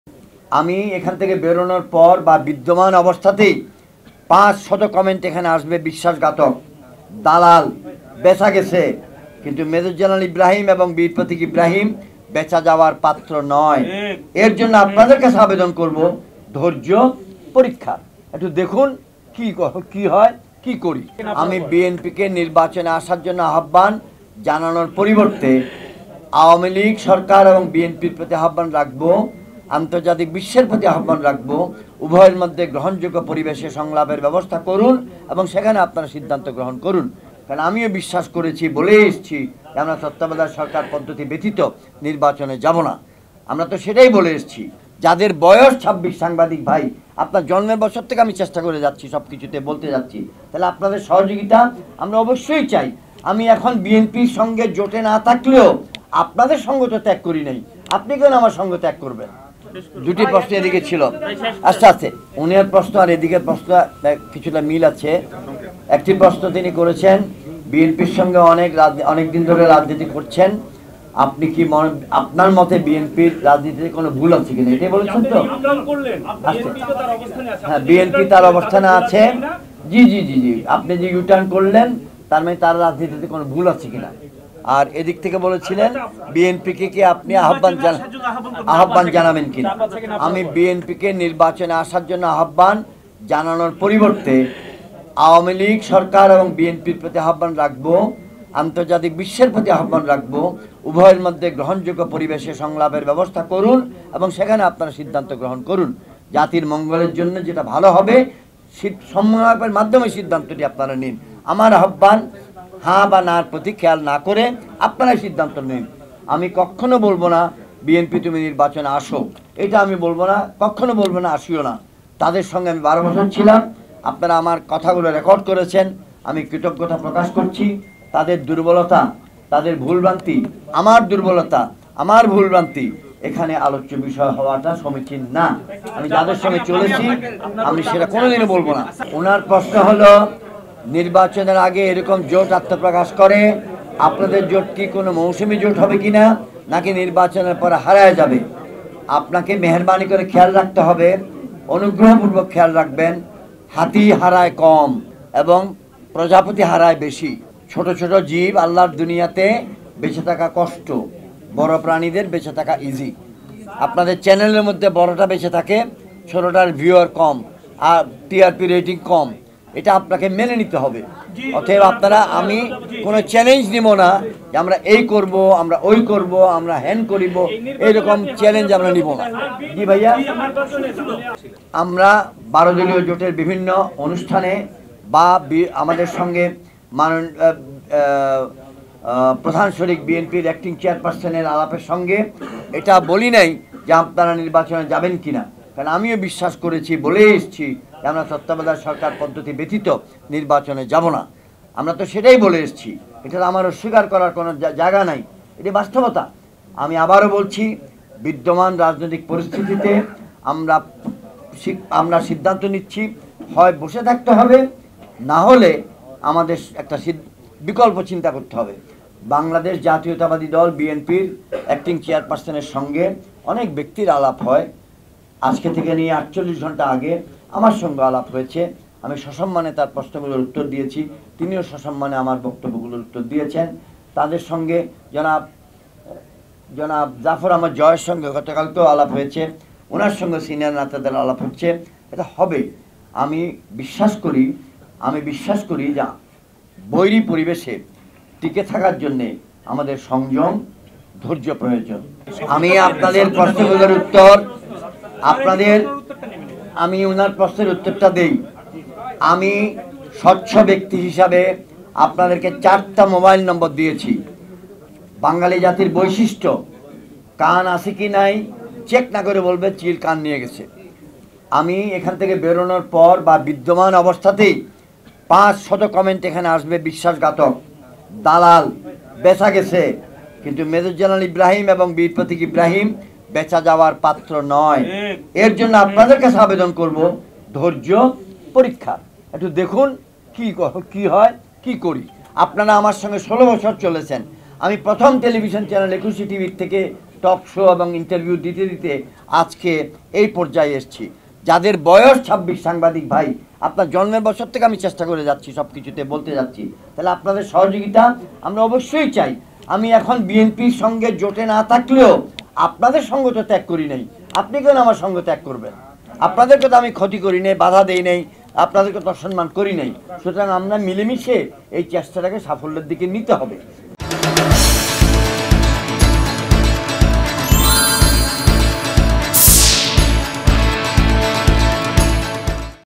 আমি বিশ্বাস ঘাতক, দালাল, বেছা গেছে, মেজর ইব্রাহিম বিক্রি হয় নাই | নির্বাচনে অংশ নেবে যুক্তফ্রন্ট
আমাদের ( AM Mission TV ) চ্যানেল এর পক্ষ থেকে আপনাকে স্বাগতম। সঙ্গে থাকার জন্য ধন্যবাদ আপনাকে |